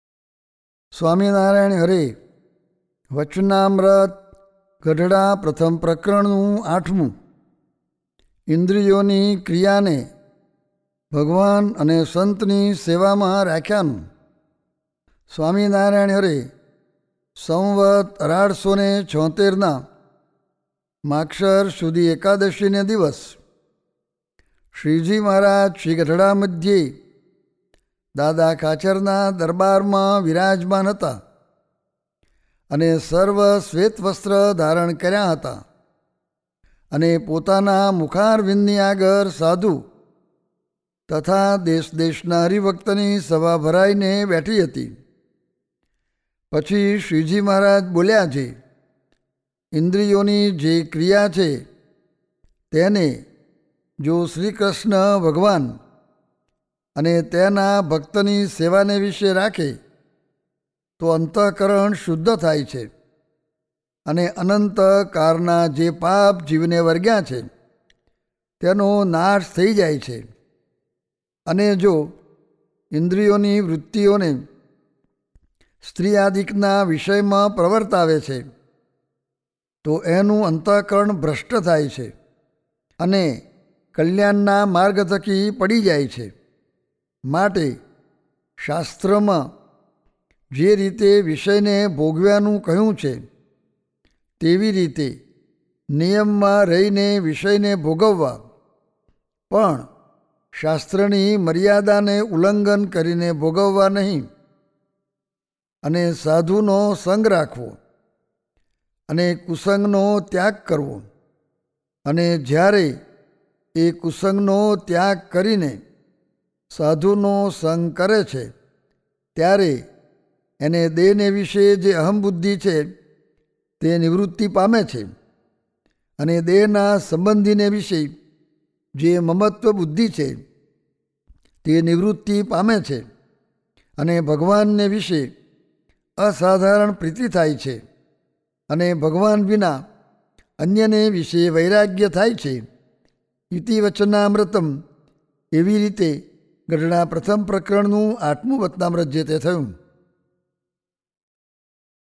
VACHANAMRUT PRATHAM 8 INDRIYONI KRIYANE BHAGWAN ANE SANTNI SEVAMA RAKHYANU
ગઢડા પ્રથમ ૮

ઇન્દ્રિયોની ક્રિયાને ભગવાન અને સંતની સેવામાં રાખ્યાનું
સંવત ૧૮૭૬ના માગશર સુદિ ૧૧ એકાદશીને દિવસ શ્રીજીમહારાજ શ્રીગઢડા મધ્યે દાદાખાચરના દરબારમાં વિરાજમાન હતા અને સર્વ શ્વેત વસ્ત્ર ધારણ કર્યાં હતાં અને પોતાના મુખારવિંદની આગળ સાધુ તથા દેશદેશના હરિભક્તની સભા ભરાઈને બેઠી હતી.

પછી શ્રીજીમહારાજ બોલ્યા જે, “ઇન્દ્રિયોની જે ક્રિયા છે તેને જો શ્રીકૃષ્ણ ભગવાન અને તેના ભક્તની સેવાને વિષે રાખે તો અંતઃકરણ શુદ્ધ થાય છે અને અનંત કાળનાં જે પાપ જીવને વળગ્યાં છે તેનો નાશ થઈ જાય છે. અને જો ઇન્દ્રિયોની વૃત્તિઓને સ્ત્રીઆદિકના વિષયમાં પ્રવર્તાવે છે તો એનું અંતઃકરણ ભ્રષ્ટ થાય છે અને કલ્યાણના માર્ગ થકી પડી જાય છે. માટે શાસ્ત્રમાં જે રીતે વિષય ભોગવ્યાનું કહ્યું છે તેવી રીતે નિયમમાં રહીને વિષયને ભોગવવા, પણ શાસ્ત્રની મર્યાદાને ઉલ્લંઘન કરીને ભોગવવા નહીં. અને સાધુનો સંગ રાખવો અને કુસંગનો ત્યાગ કરવો. અને જ્યારે એ કુસંગનો ત્યાગ કરીને સાધુનો સંગ કરે છે ત્યારે એને દેહને વિષે જે અહંબુદ્ધિ છે તે નિવૃત્તિ પામે છે અને દેહના સંબંધીને વિષે જે મમત્વબુદ્ધિ છે તે નિવૃત્તિ પામે છે અને ભગવાનને વિષે અસાધારણ પ્રીતિ થાય છે અને ભગવાન વિના અન્યને વિષે વૈરાગ્ય થાય છે.”

॥ ઇતિ વચનામૃતમ્ ॥ ૮ ॥